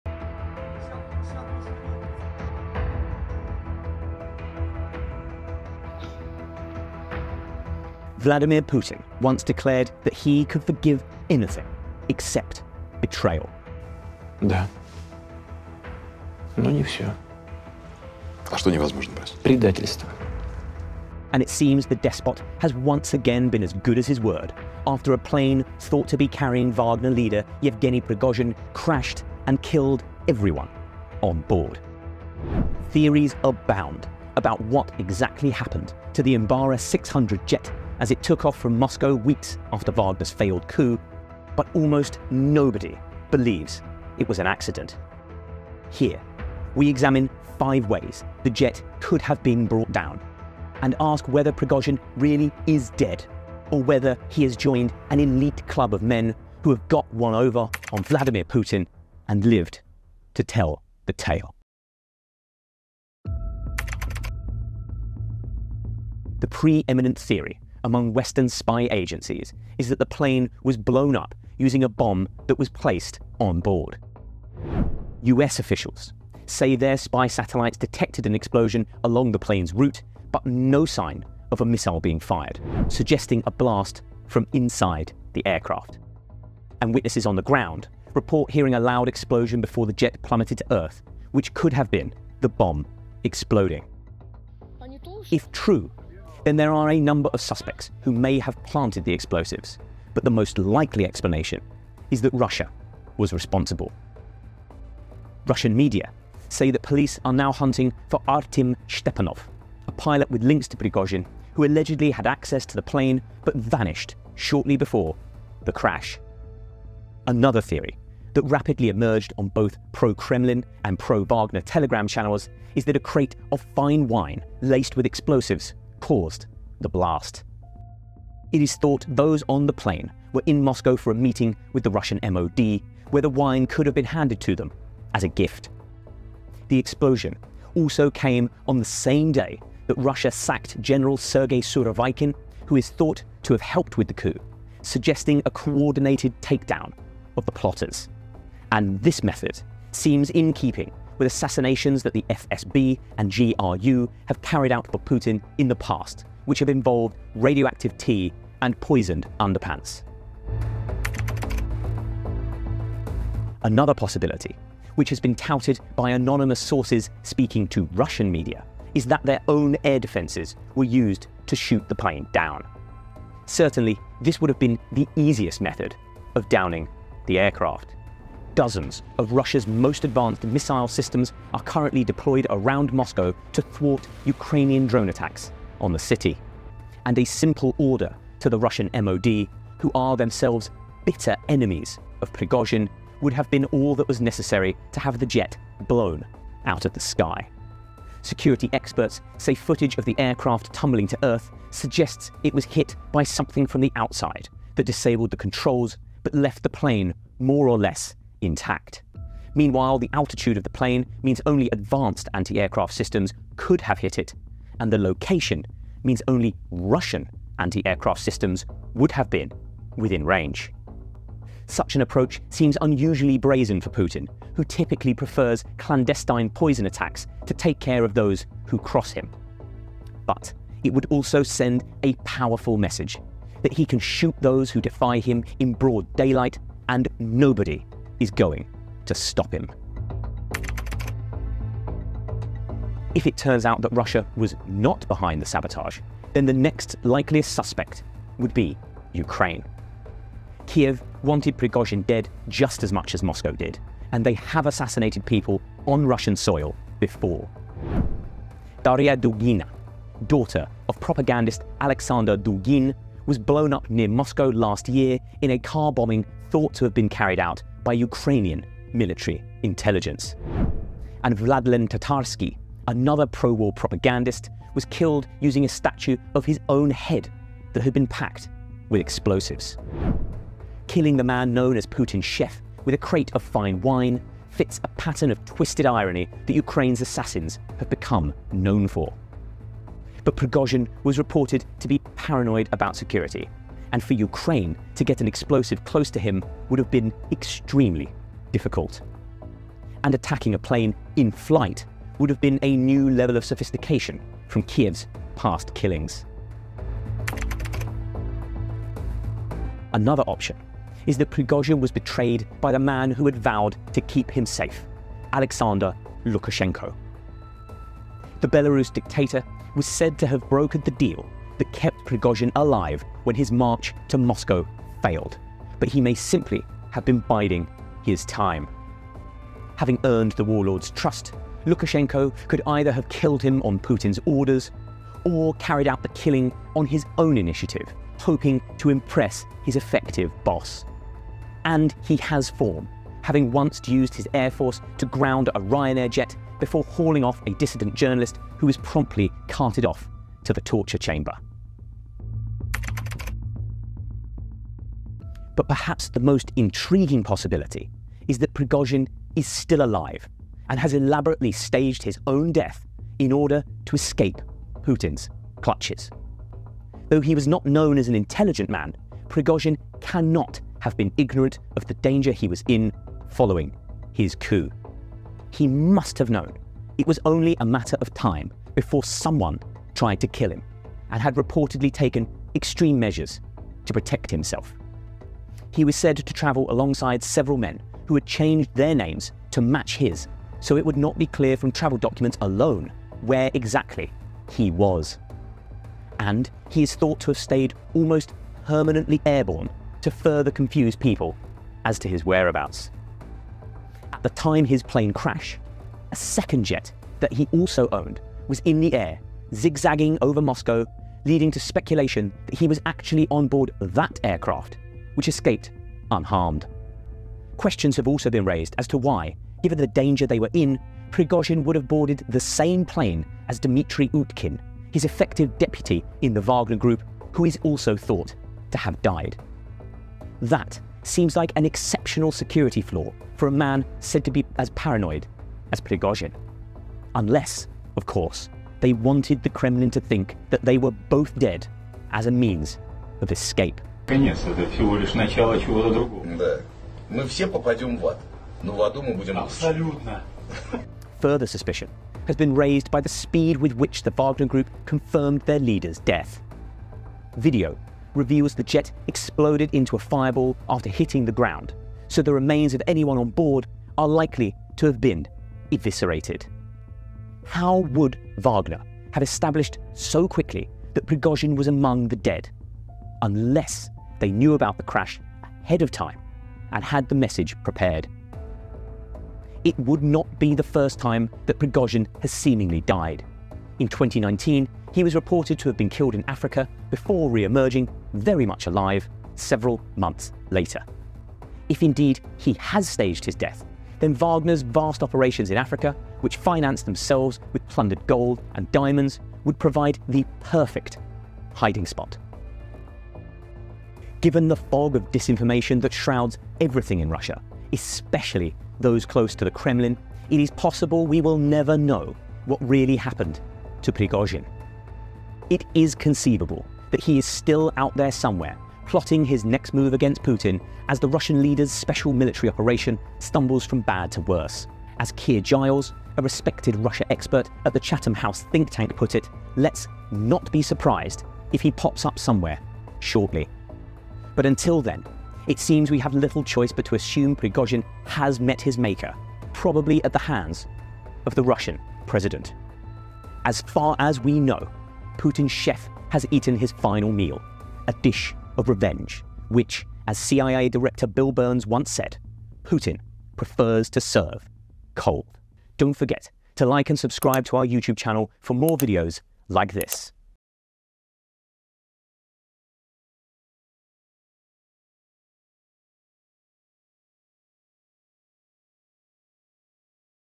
Yevgeny Prigozhin: Five ways Putin could have blown his plane up | Russia Ukraine update
Vladimir Putin once declared that he could forgive anything except betrayal. And it seems the despot has once again been as good as his word, after a plane thought to be carrying Wagner leader Yevgeny Prigozhin crashed and killed everyone on board. Theories abound about what exactly happened to the Embarer 600 jet as it took off from Moscow weeks after Wagner’s failed coup, but almost nobody believes it was an accident. Here, we examine five ways the jet could have been brought down. And ask whether Prigozhin really is dead, or whether he has joined an elite club of men who got one over on Putin and lived to tell the tale.